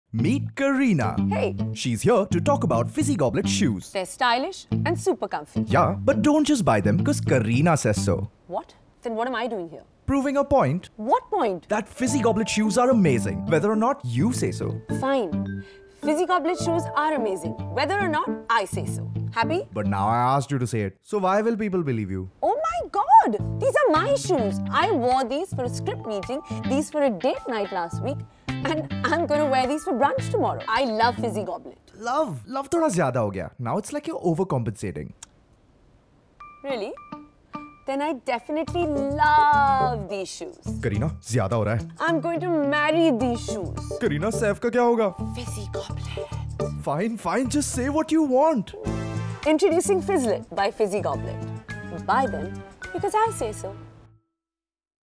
Kareena Kapoor Khan introduces Fizzlet by Fizzy Goblet
Kareena LOOOOVES these shoes & you will too. Introducing Fizzlet - the cool, carefree cousin of Fizzy Goblet. Explore 33 new designs now! 🤩